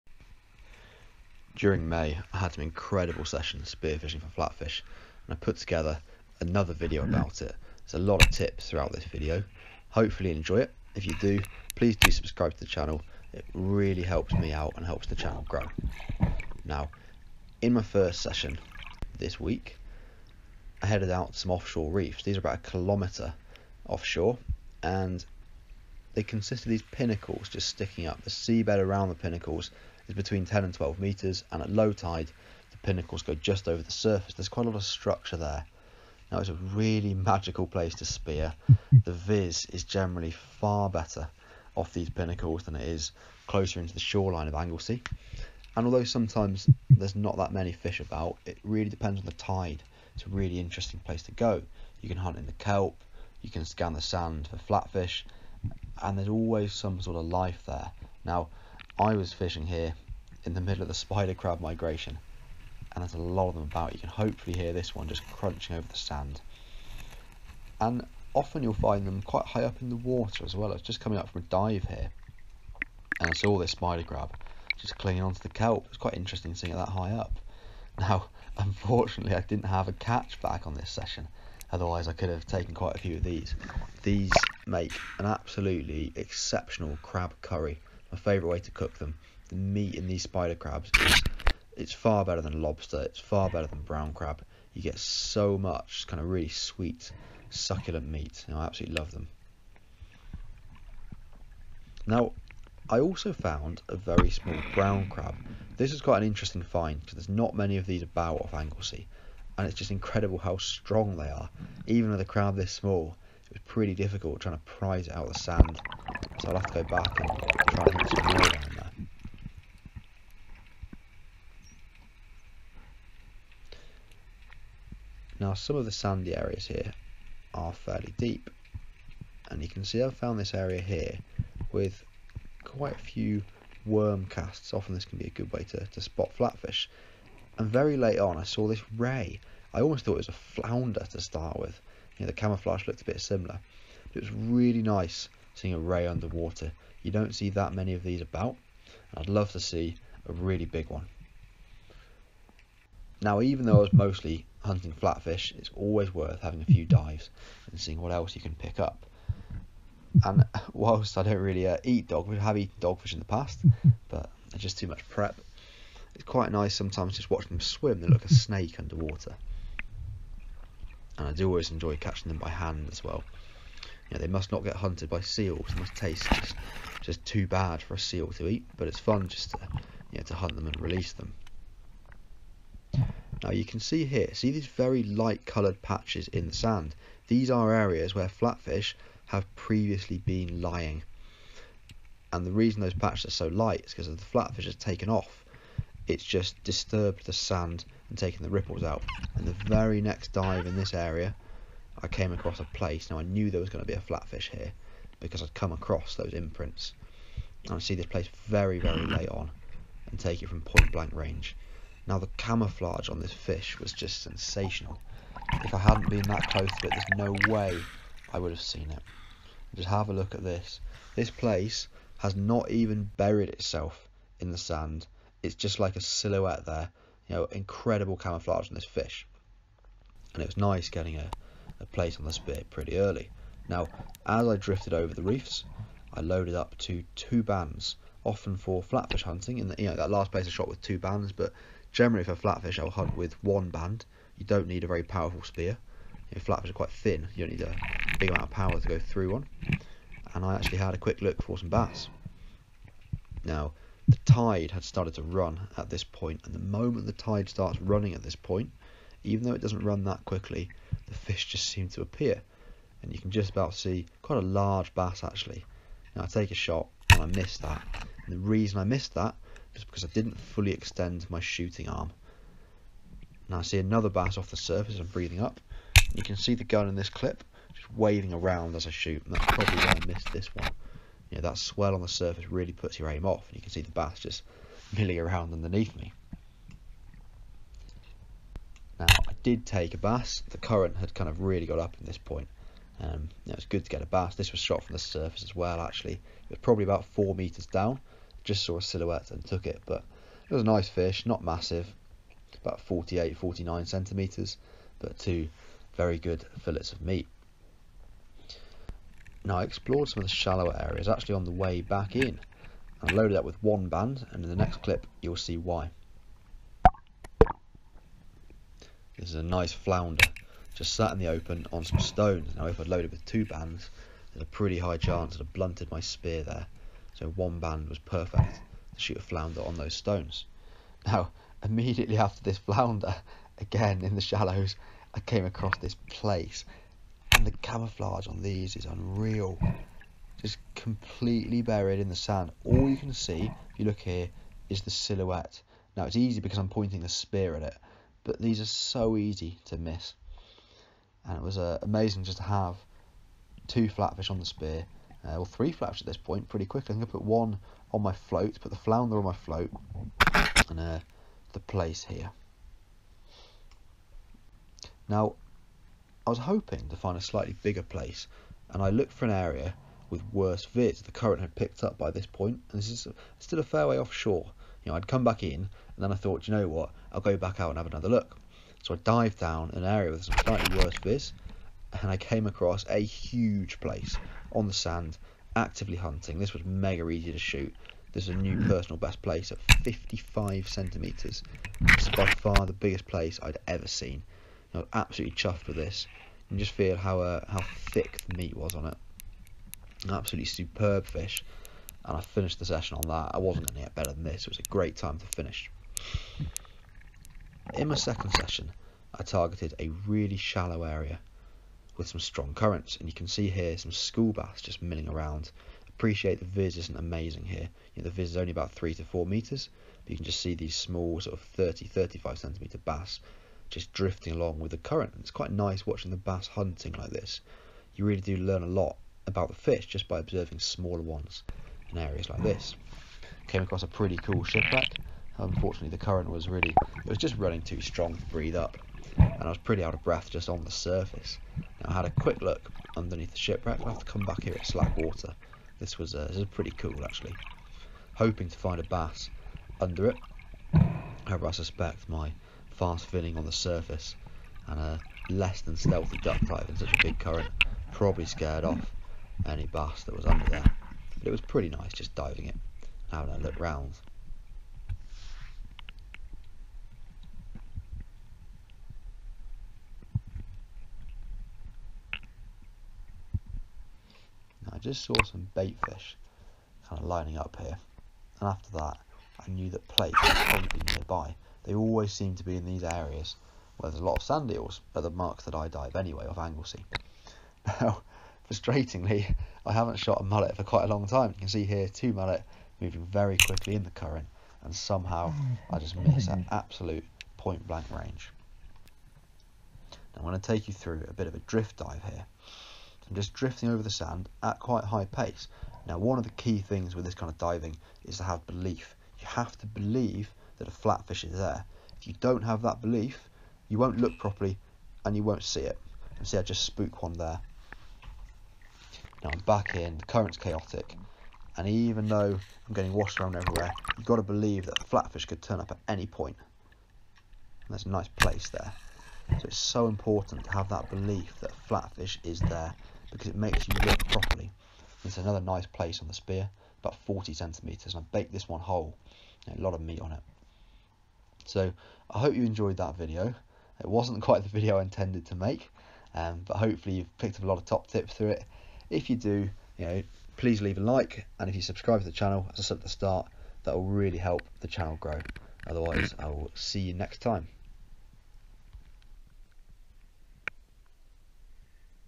Spearfishing for big Plaice from remote offshore reefs (North Wales Spearfishing)
Late spring can be the ideal time to spearfish for flatfish off the coast of North Wales, large numbers of Plaice arrive inshore and there is always the chance of shooting a Sea Bass at the same marks.

In this video I head to some remote offshore reefs, a series of rocky pinnacles that rise up from the seabed and attract large numbers of fish of all species.

Lots more top tips on spearfishing for flatfish, in this video a good number of Plaice and flounder are shot, plus footage of Thornback Rays and a few other species.

North Wales Spearfishing is truly excellent, with a vast number of exciting marks to try and a range of species, if you enjoy the video, like and subscribe to see more content like this!